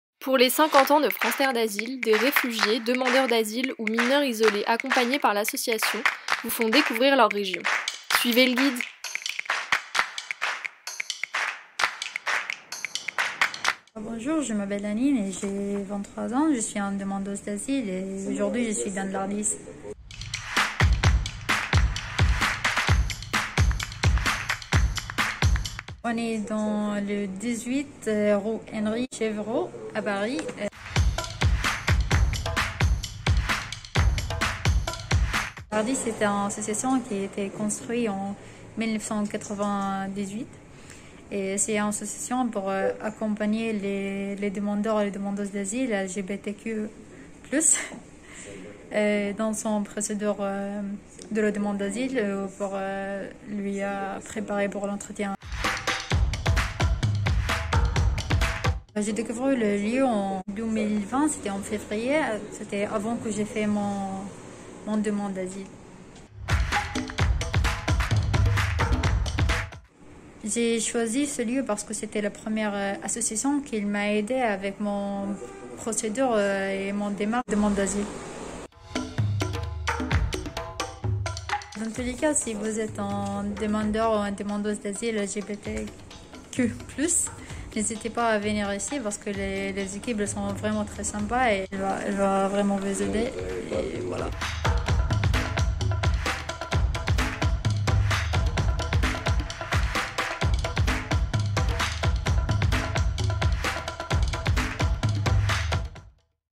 Suivez le guide ! avec Haneen
Cet été, voyagez en France avec les résidents de nos établissements ! Aujourd’hui, Haneen vous fait découvrir l'ARDHIS à Paris.